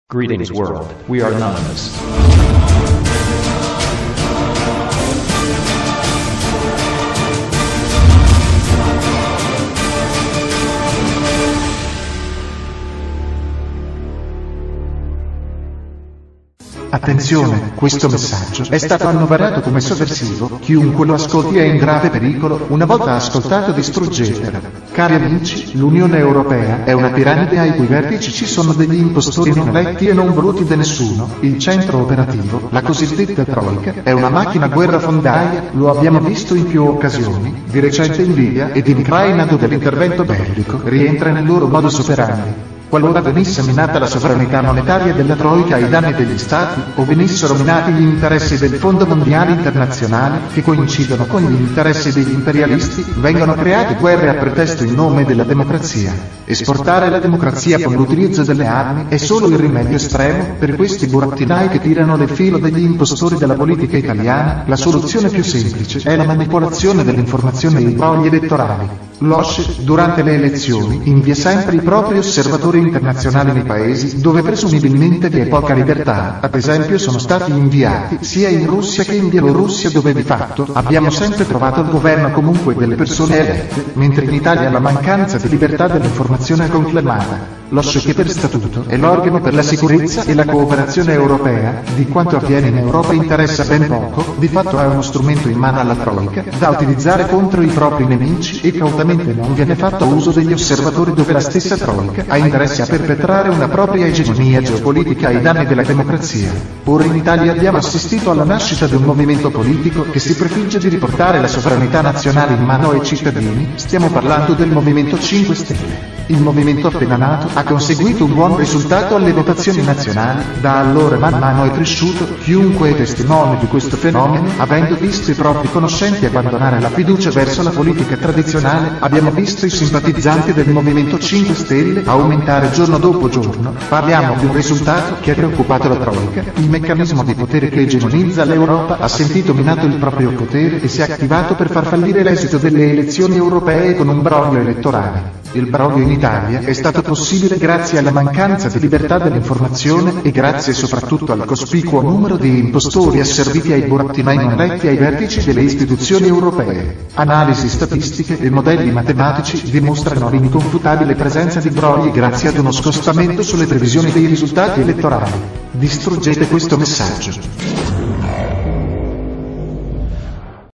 Brogli elezioni europee 2014
Anonymous segnala la presenza di brogli alle elezioni europee. Il risultato elettorale PD, M5S è stato artificiosamente salsato grazie alla presenza di brogli. Barroso, troika, brogli. UE elezioni, flop m5s.